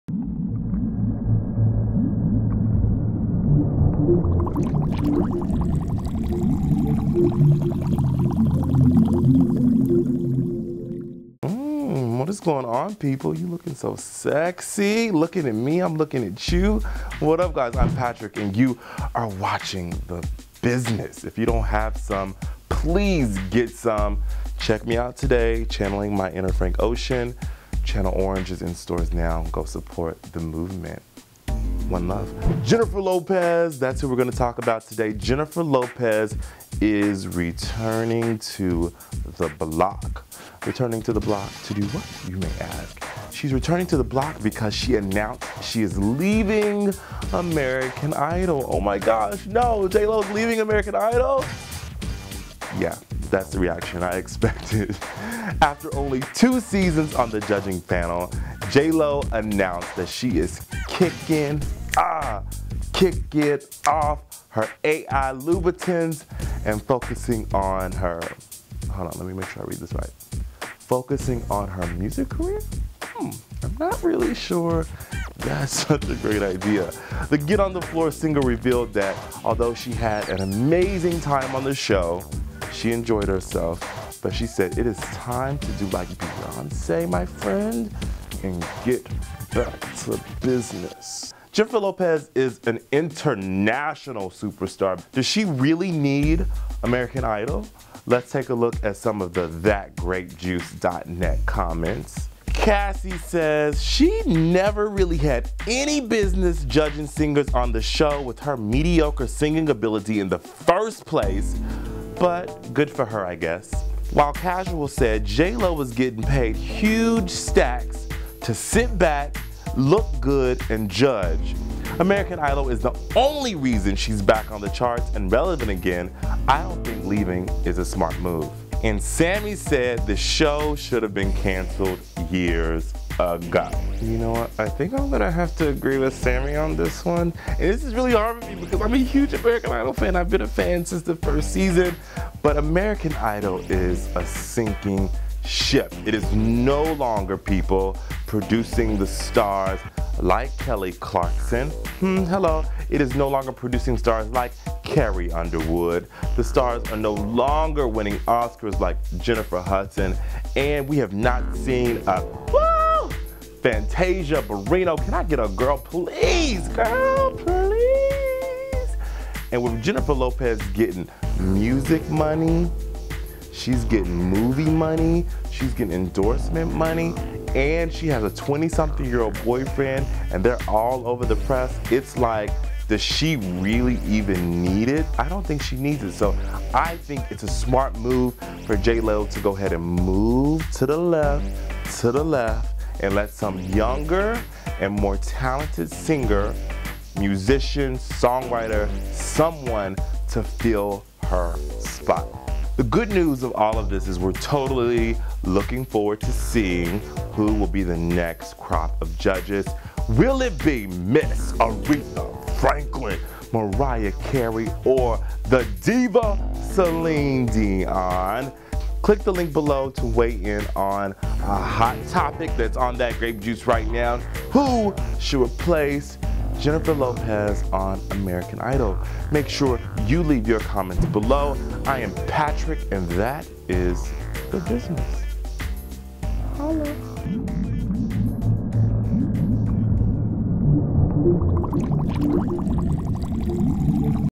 American Idol: Should Jennifer Lopez Have Left?
J.Lo Leaves American Idol: That Grape Juice weighs-in on Jennifer Lopez's decision to leave the hit show. Was it a good or bad career move?